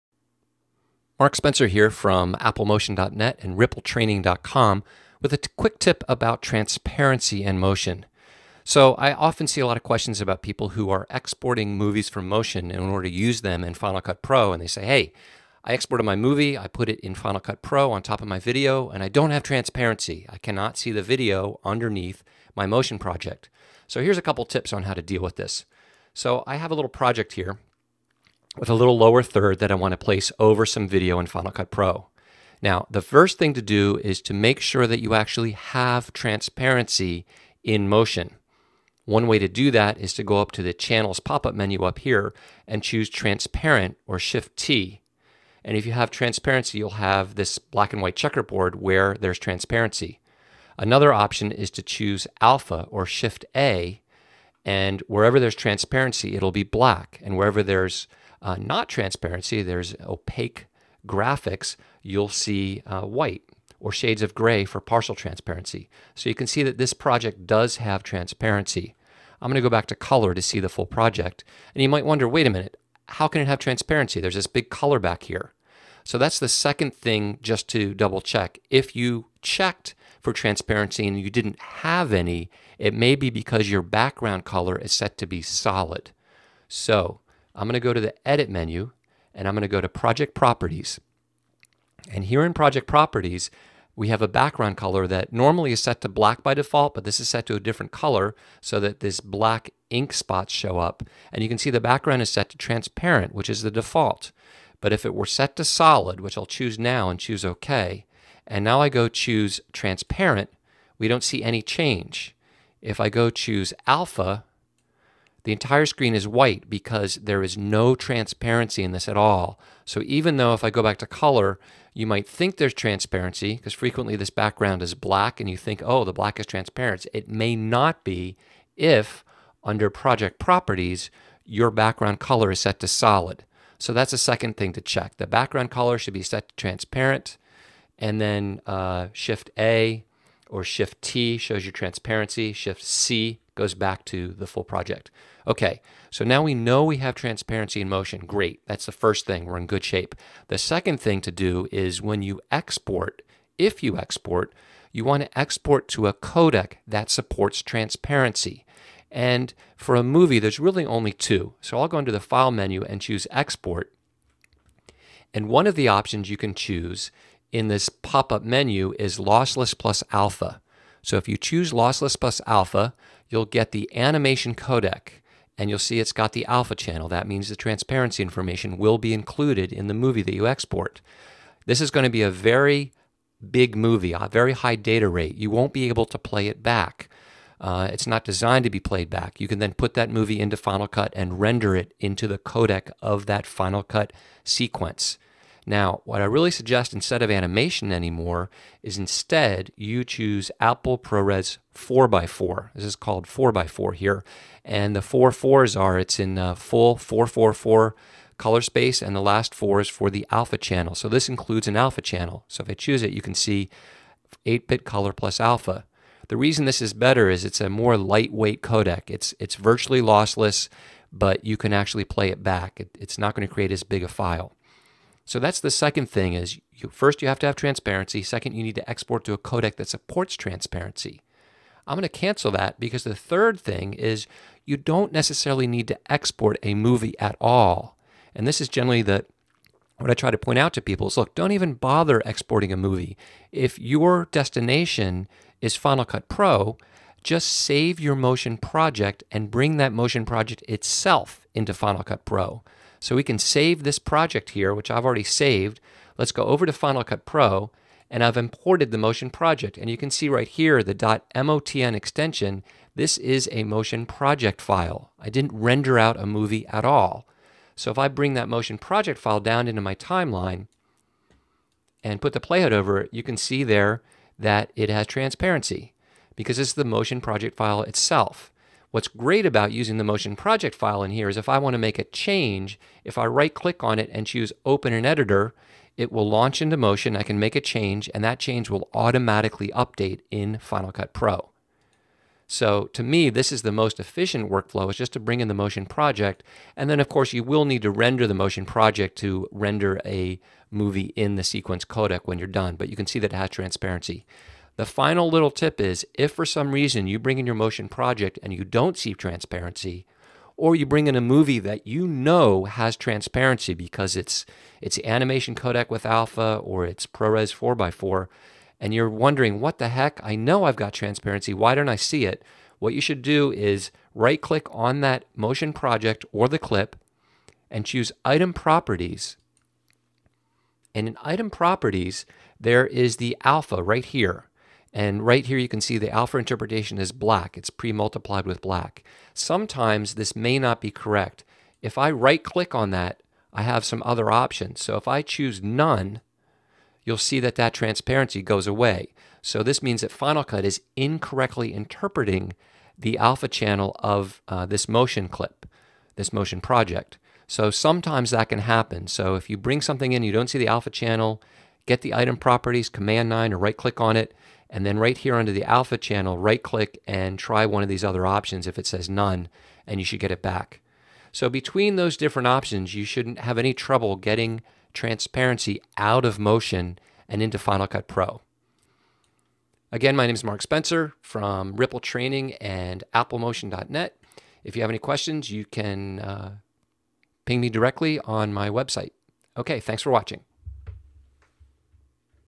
Mark Spencer's Motion Tip: Working with Transparency
Having trouble exporting a movie from Motion for Final Cut Pro with transparency? Here are a few tips on how to make sure everything works smoothly.